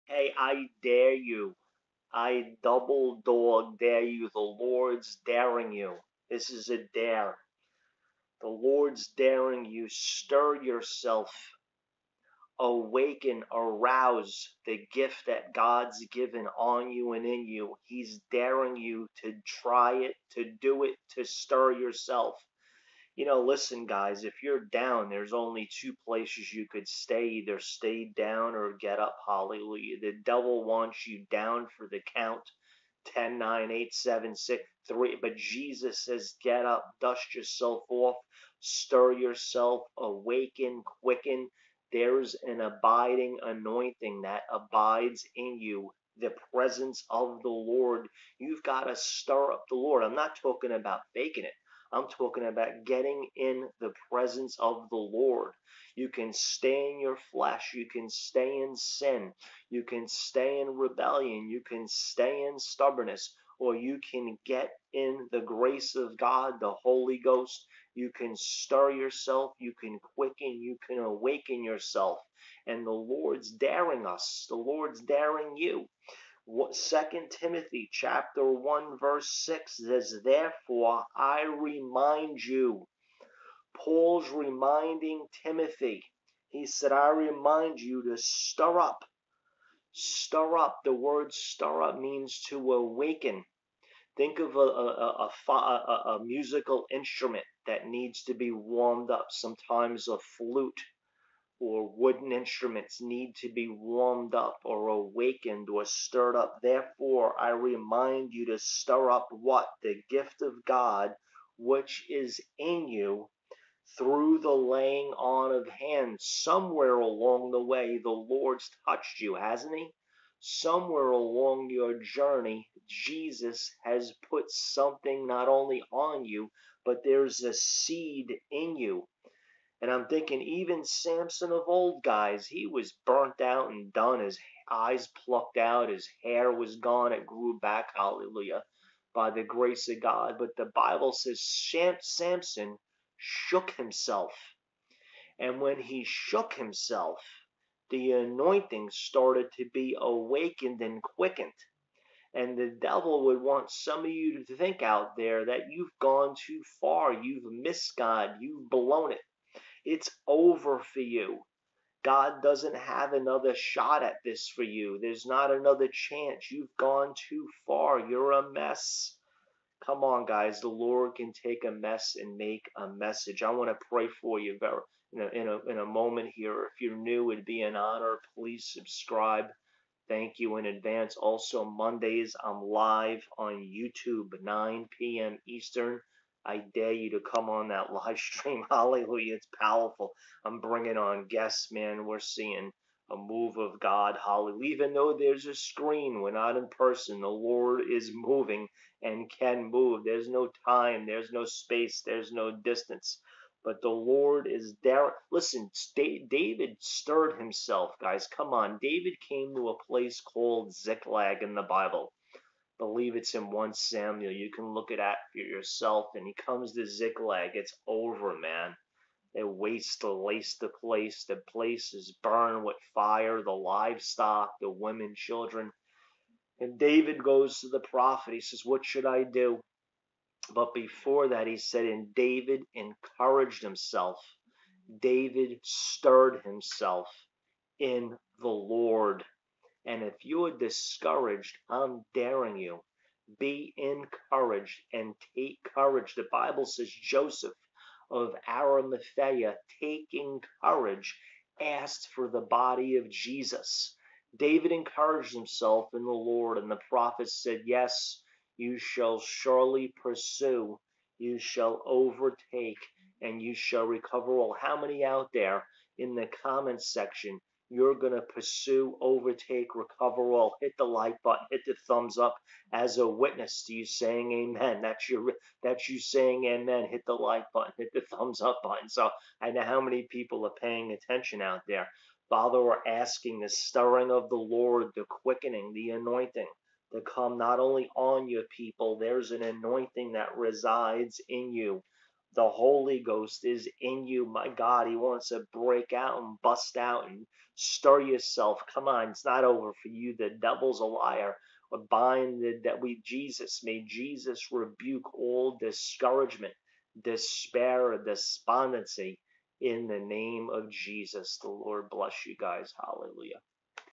Prophetic word the LORD SAYS I DARE YOU TO STIR YOURSELF AND SEE WHAT HAPPENS!!!
Robb shares on the stirring of the lord